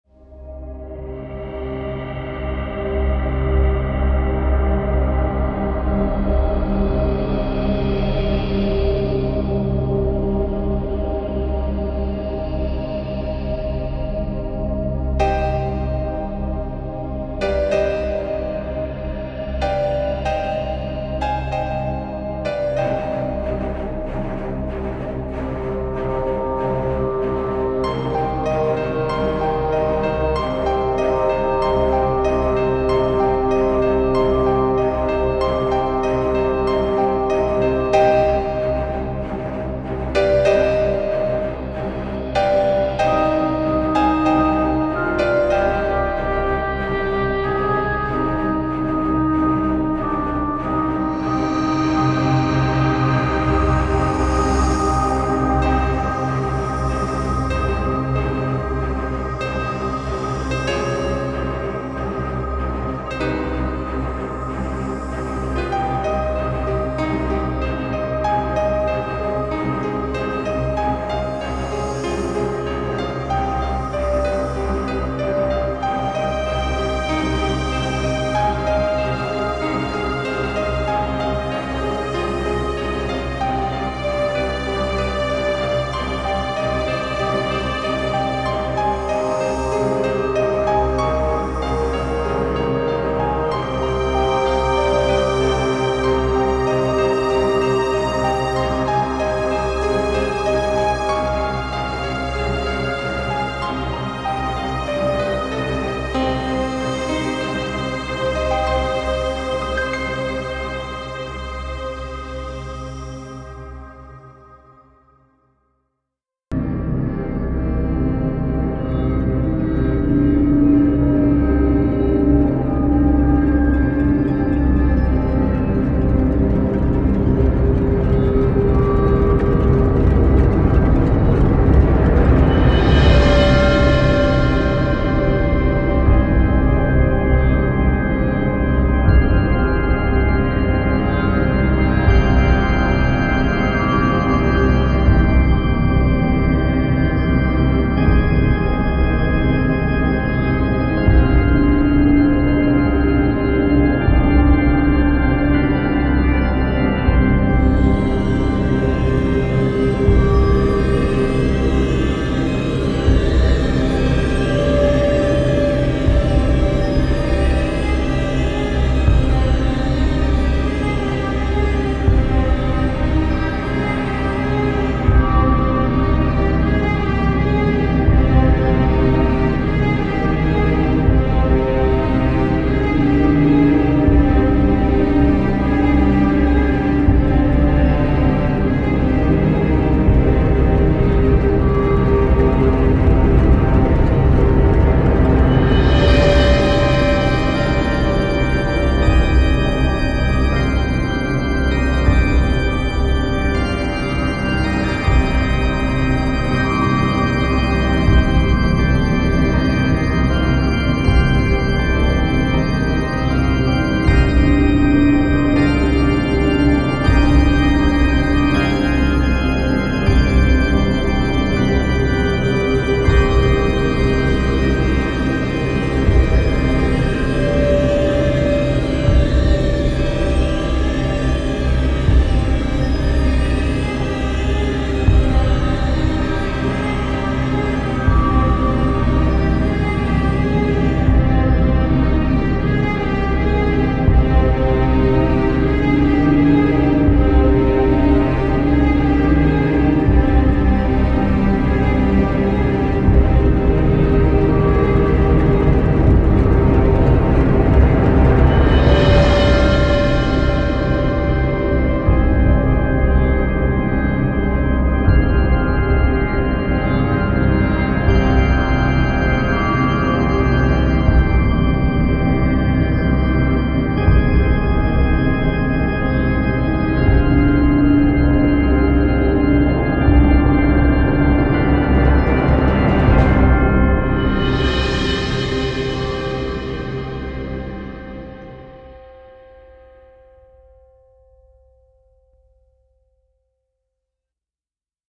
Blender 3D - Glowing Magic Ring (Procedural Shader Nodes Material)
Timelapse of working on Glowing Magic Circle in Blender 3D